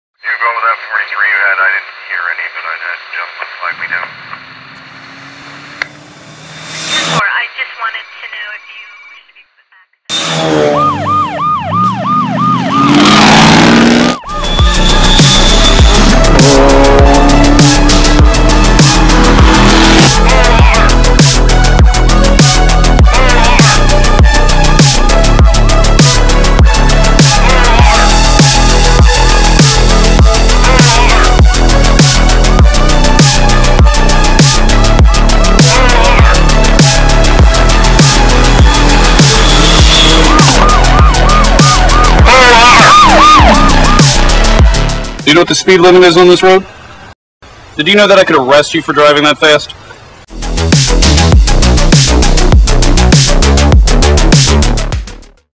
Chris Snow | Ford Focus RS
Quick short of Chris Snows Ford Focus Rs, did a little smokie and the bandit spoof on this one. Wasn't planned spur of the moment was just out for photos, but when we seen our buddy Connor out with his old interceptor we knew we had to shoot something. Enjoy!